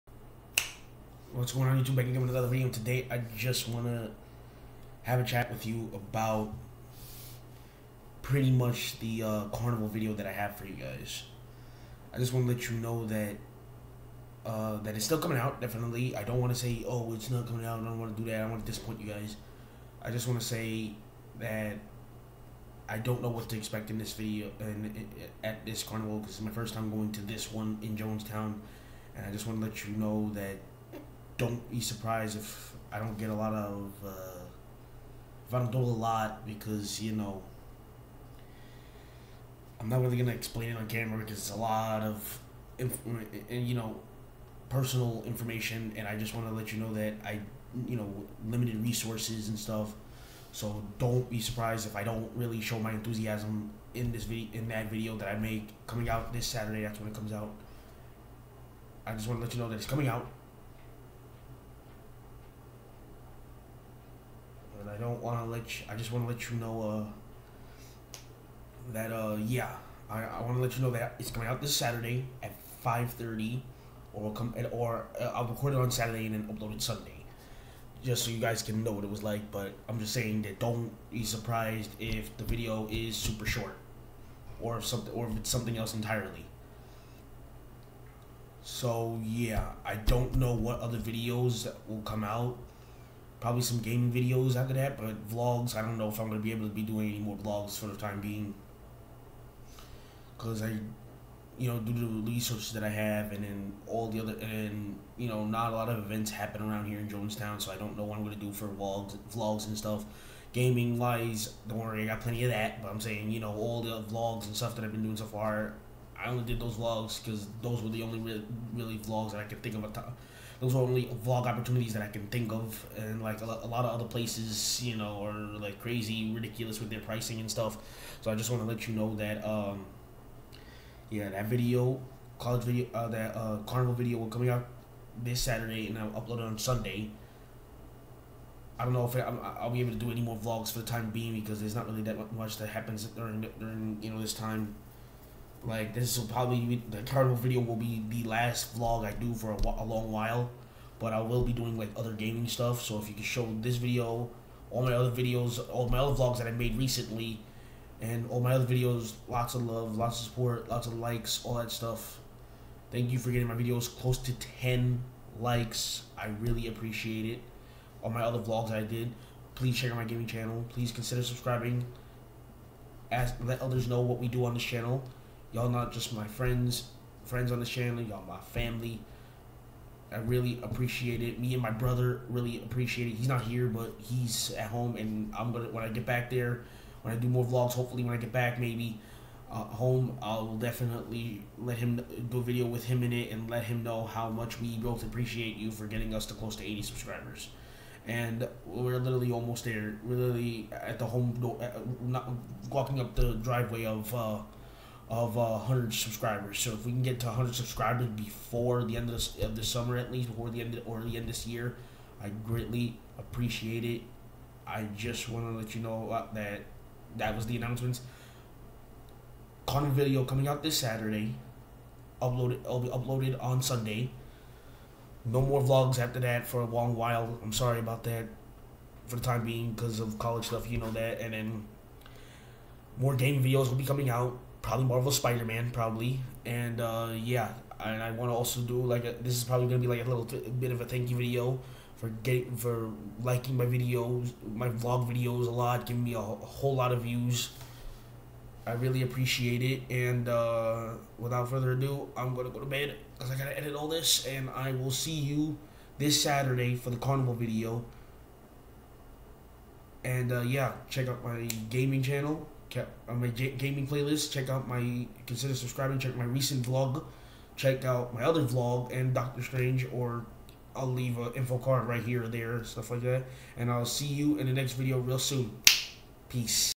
The plan about the carnival vlog video and future vlogs in general 👍😢🥰😊🤓🏷️🏬🤔🥳😔😐
Thank you guys for liking my shopping video and five guys video and showing it lots of love and support please understand my whole video this video and dont worry carnival video coming Sunday